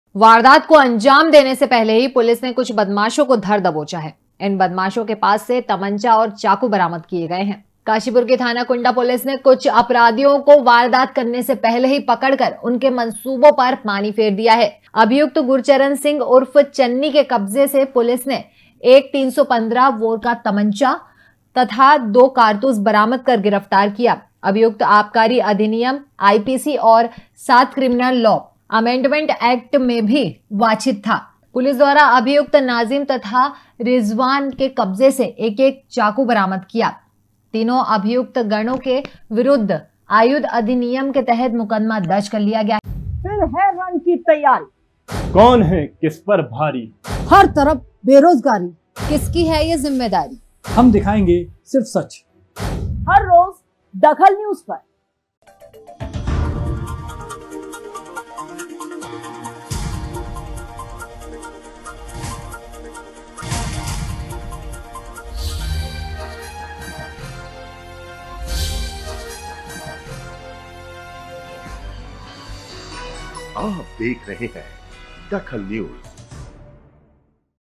DAKHAL NEWS अवैध तमंचे और चाकू वाले बदमाश पकड़े गए
अवैध तमंचे और चाकू वाले बदमाश पकड़े गए
वारदात करने से पहले पुलिस ने किया गिरफ्तार